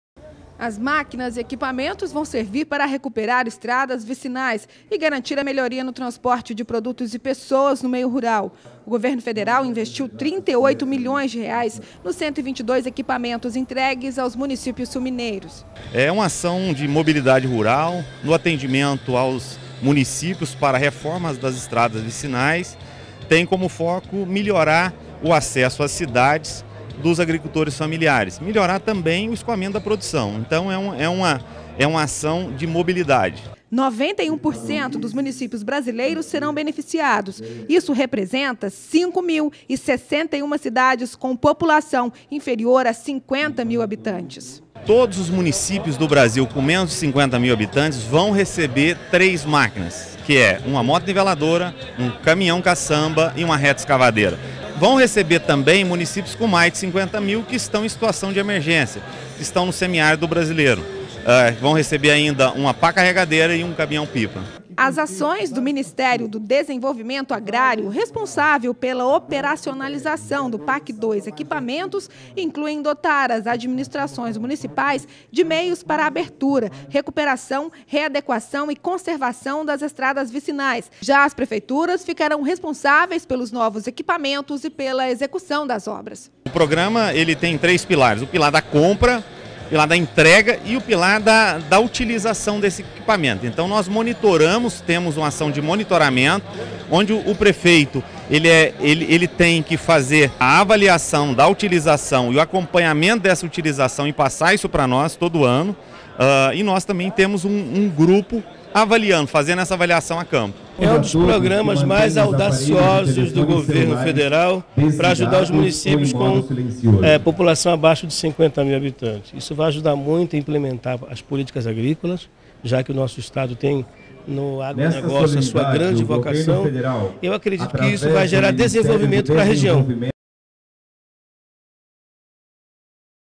Governo Federal entrega máquinas agrícolas para 122 cidades da região
Em 2010 o Governo Federal lançou a 2ª etapa do Programa de Aceleração do Crescimento, o chamado PAC 2. Nesta fase o programa tem propostas novas. Entre elas está a expansão das malhas rodoviárias e a conservação das estradas vicinais, importantes para o escoamento da produção e para a segurança do tráfego nos pequenos municípios. Nesse sentido, o Ministério do Desenvolvimento Agrário vem adquirindo máquinas para a recuperação de estradas vicinais. E na manhã de hoje aconteceu em Pouso Alegre a solenidade de entrega de 98 caminhões caçambas e 32 motoniveladoras.